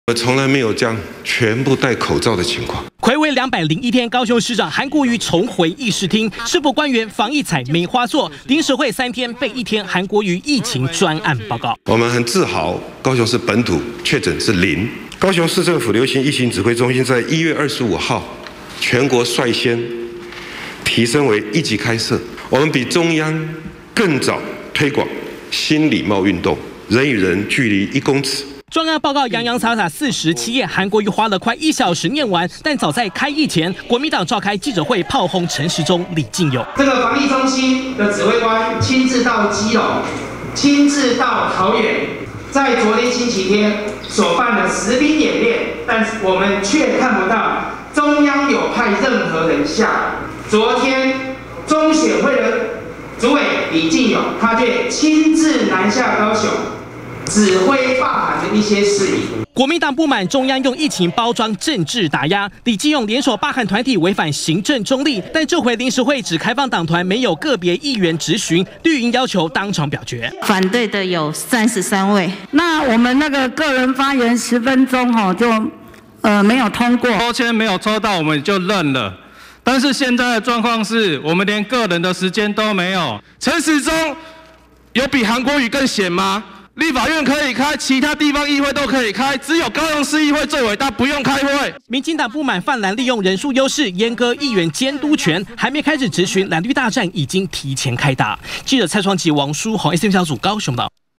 20200504中天新聞 韓國瑜疫情專案報告 綠嗆閹割個人質詢10分鐘
高雄市議會，臨時會今天登場，市長韓國瑜列席疫情專案報告，報告之前國民黨召開記者會，砲轟疫情指揮中心指揮官陳時中，和中選會主委李進勇，對韓國瑜政治鬥爭打壓，另外這回臨時會，從3天變成1天，只有黨團質詢，沒有個別議員質詢時間，綠營大酸韓國瑜很閒，高雄市議會最偉大不用開會。